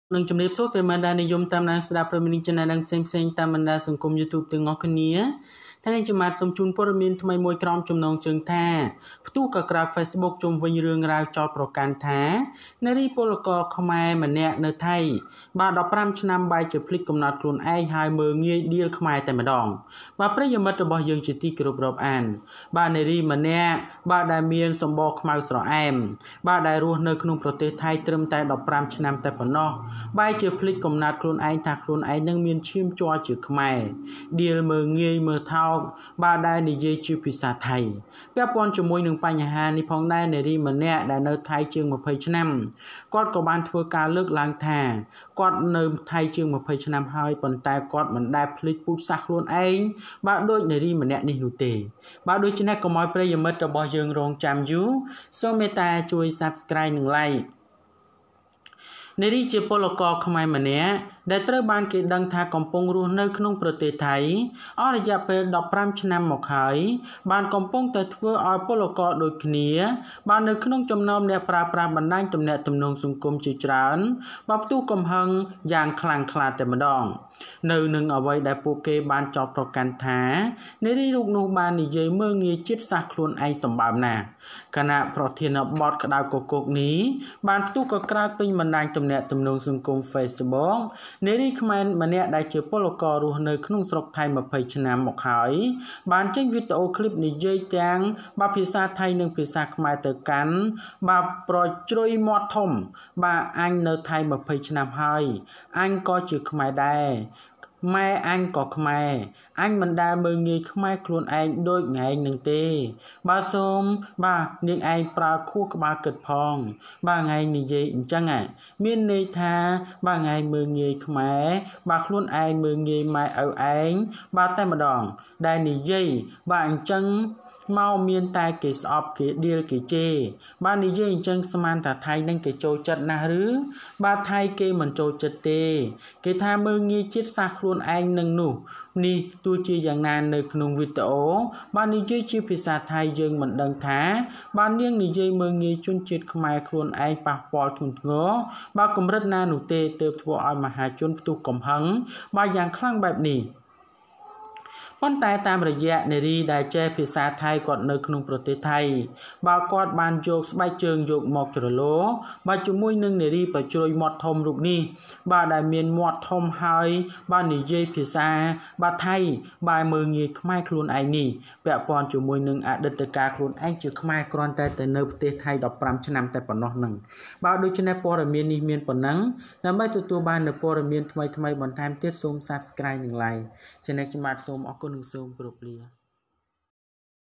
ផ្ទុះ​កក្រើក​ហ្វេ​ស​ប៊ុ​ក​! ជុំវិញ​រឿងរ៉ាវ​ចោទប្រកាន់ថា នារី​ពលករ​ខ្មែរ​ម្នា​ក់នៅ​ថៃ​១៥​ឆ្នាំ បែរជាភ
ផ្ទុះ​កក្រើក​ហ្វេ​ស​ប៊ុ​ក​! ជុំវិញ​រឿងរ៉ាវ​ចោទប្រកាន់ថា នារី​ពលករ​ខ្មែរ​ម្នា​ក់នៅ​ថៃ​១៥​ឆ្នាំ បែរជាភ្លេចកំណើតខ្លួន Explosion Facebook!  It is alleged that a 15-year-old Thai woman forgot to give birth.